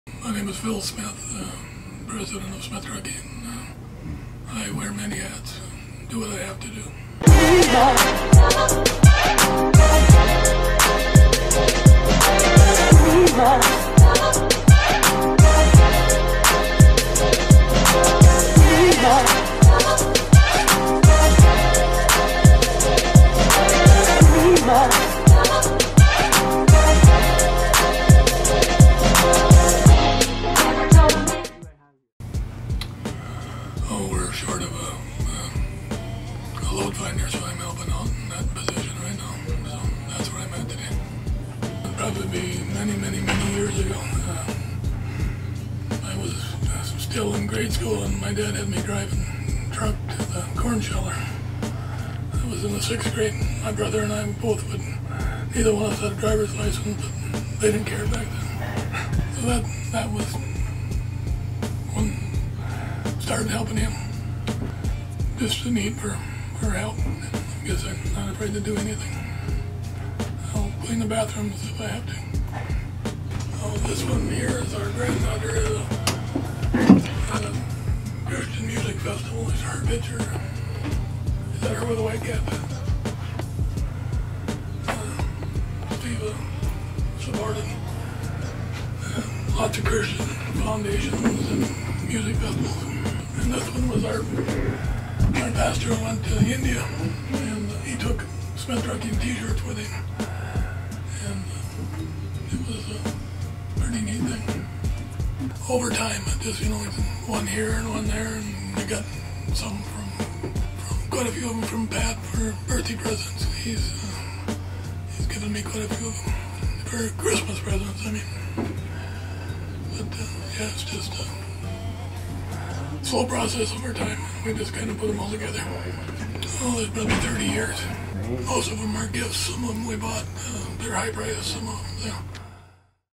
The Faces Behind Smith Trucking: Phil Smith
Get to know the faces behind Smith Trucking! In today's episode, we have the president of Smith Trucking, Phil Smith.Phil continues on the legacy of his father by taking over the company. Watch the video and learn more about how Phil operates on a day to day basis. He's an awesome dude!